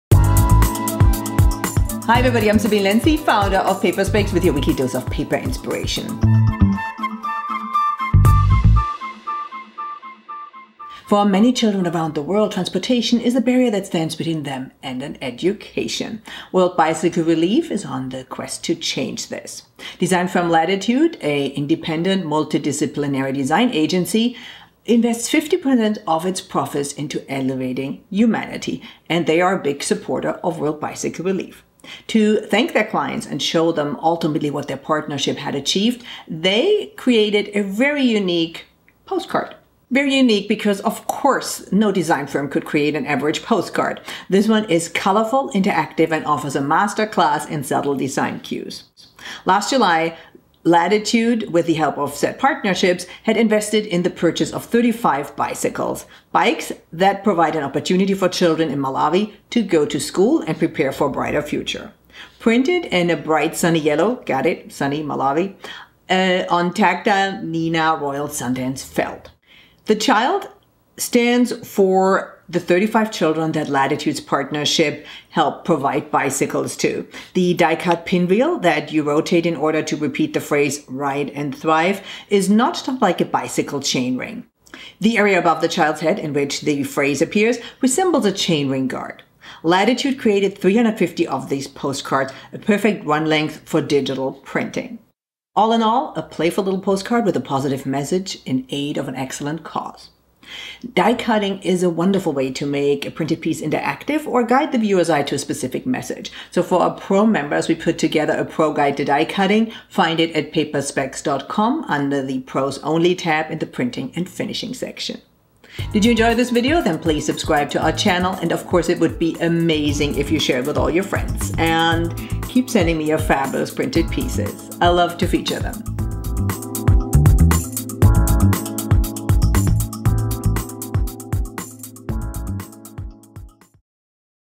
PaperSpecs.com | Paper Inspiration #301: Latitude Postcard
Sometimes you come across a piece that simply makes you smile. Is that a big thing, you ask. These days? I'd say so, wouldn't you? And this postcard crafted by brand design firm Latitude, does the trick very nicely with a warm, pleasing color on luxurious-feeling Neenah Royal Sundance Felt. Developed to celebrate Latitude's work with World Bicycle Relief – a charity that seeks to bring people in the developing world a fast and inexpensive means of reliable transportation – the postcard offers a masterclass in subtle design cues.

The child stands for the 35 children that Latitude's partnership with WBR helped provide bicycles to – a means to help them get to school. The die-cut pinwheel that you rotate in order to repeat the phrase "ride and thrive" is notched like a bicycle chainring. The area above the child's head in which the phrase appears resembles a chainring guard.

All in all a gorgeous, playful little postcard with a positive message in aid of an excellent cause. More life-affirming print pieces, please :-)